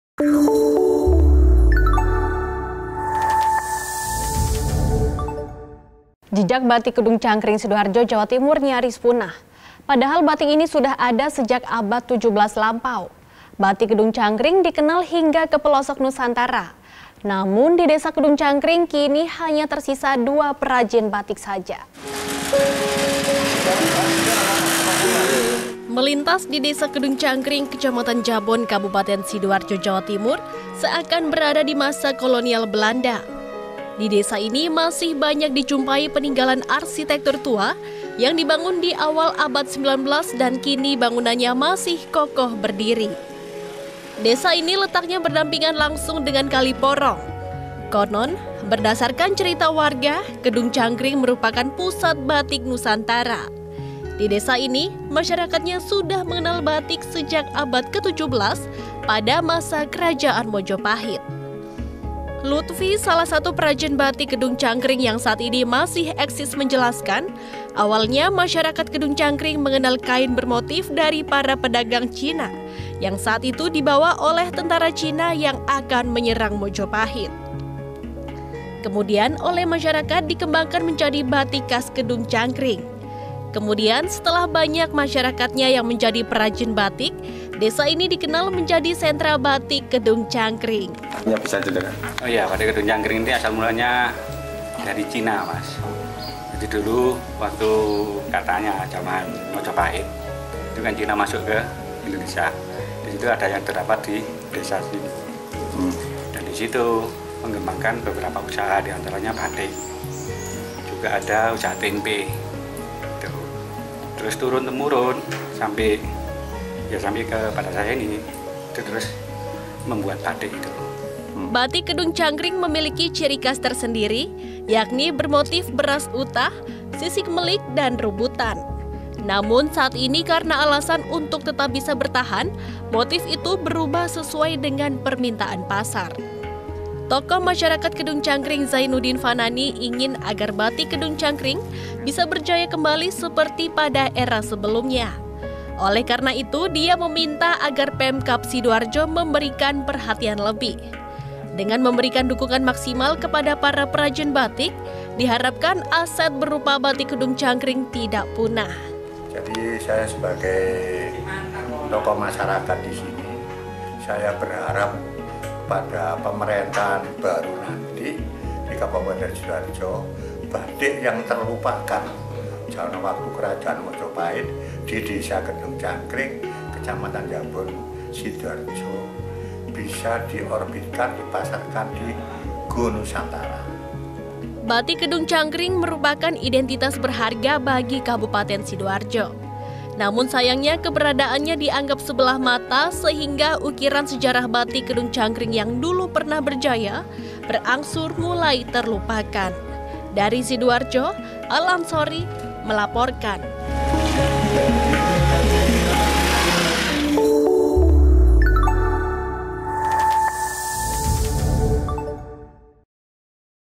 Batik Kedung Cangkring Nyaris Punah
Jejak Batik Kedung Cangkring Sidoarjo, Jawa Timur nyaris punah. Padahal batik ini sudah ada sejak abad 17 lampau. Batik Kedung Cangkring dikenal hingga ke pelosok nusantara. Namun di Desa Kedung Cankring kini hanya tersisa dua perajin batik saja.